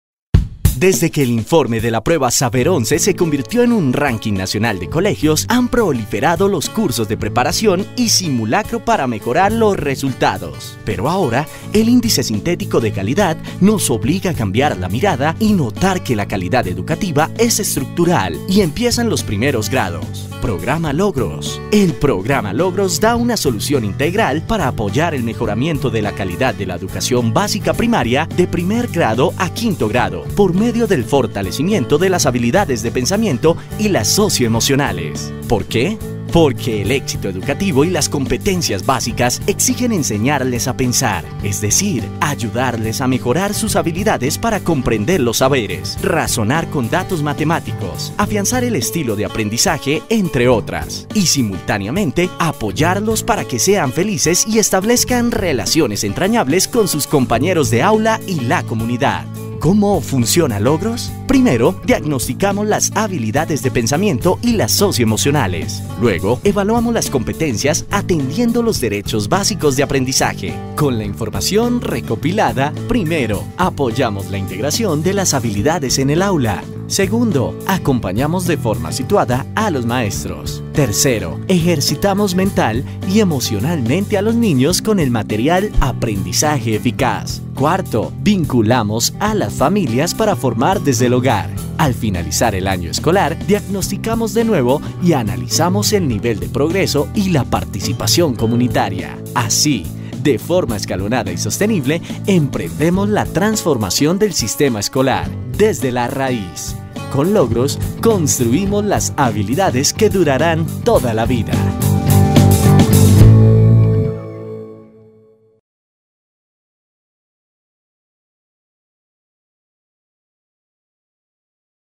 Logros
Logros es una solución educativa integral para que los educadores, los directivos docentes, las familias, los orientadores escolares y, especialmente, los niños y niñas, emprendan una ruta de aprendizaje para mejorar la calidad de la educación básica primaria.